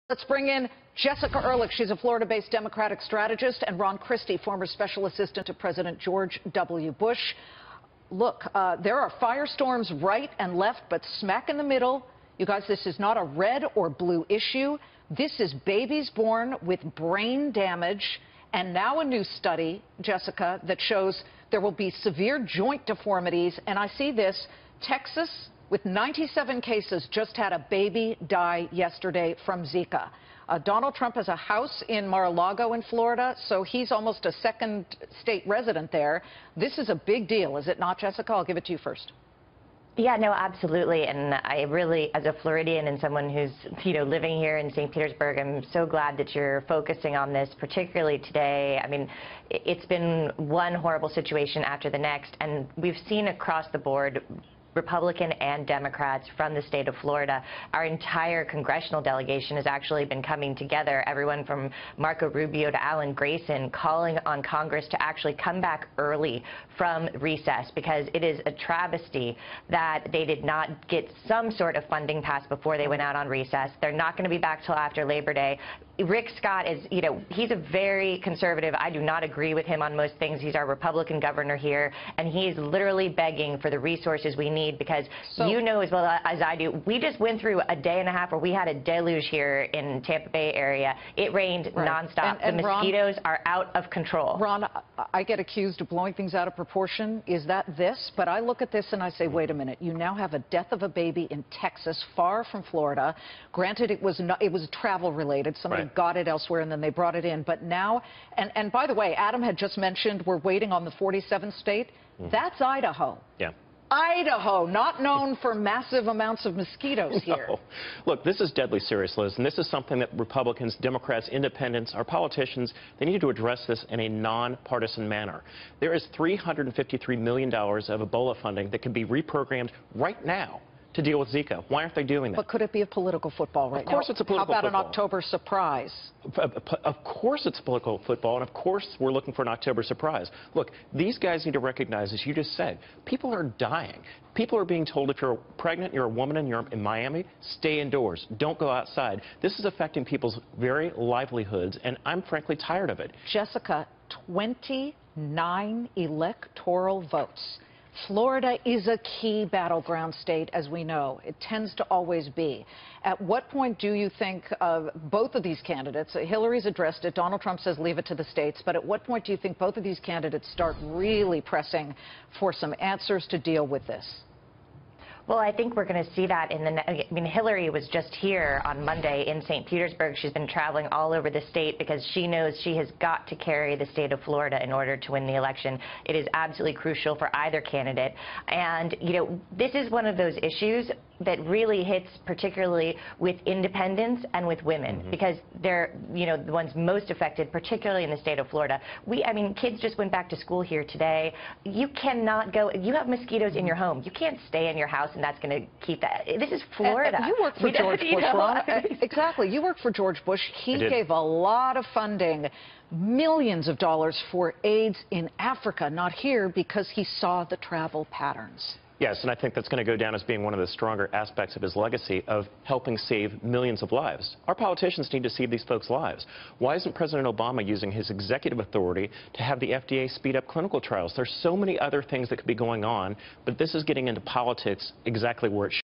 Is politics interfering with a Zika cure?
Democratic Strategist Jessica Ehrlich and Republican Strategist Ron Christie discuss how politics could be getting in the way of a cure for the Zika virus.